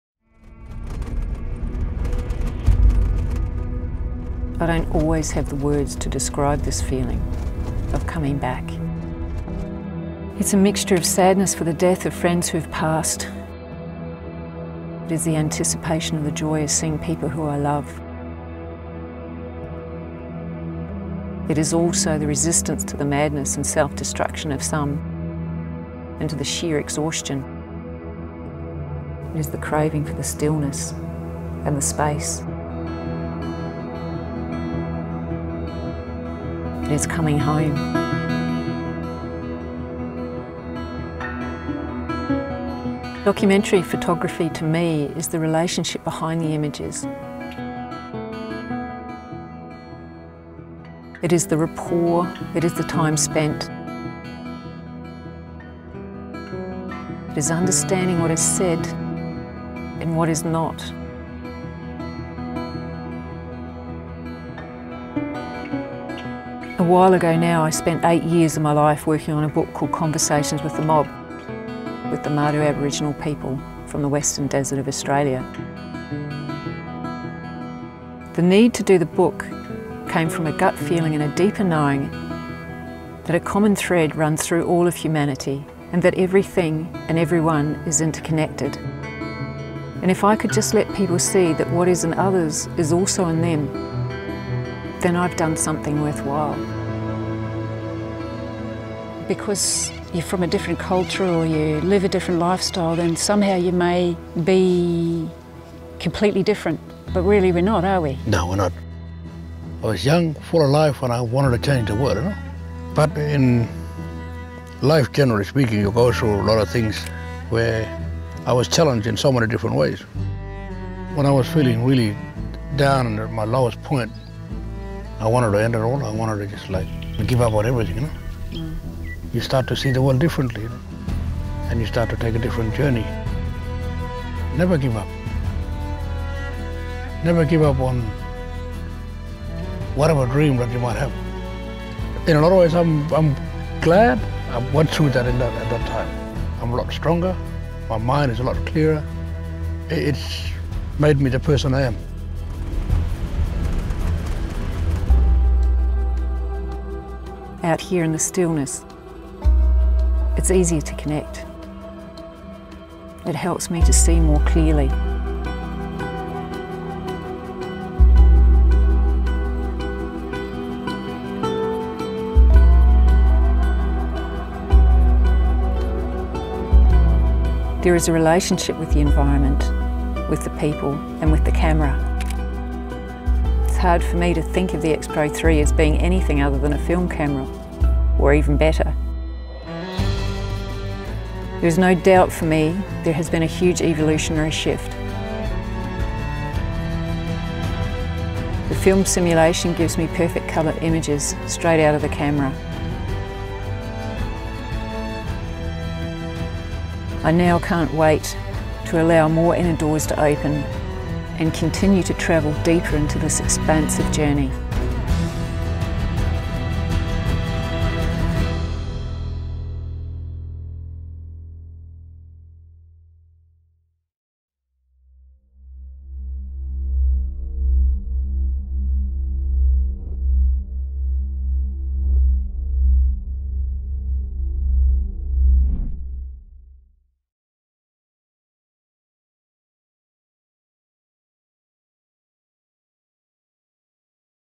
Megan Lewis x X-Pro3 / FUJIFILM
X-Photographer Megan Lewis from Australia shoots on X-Pro3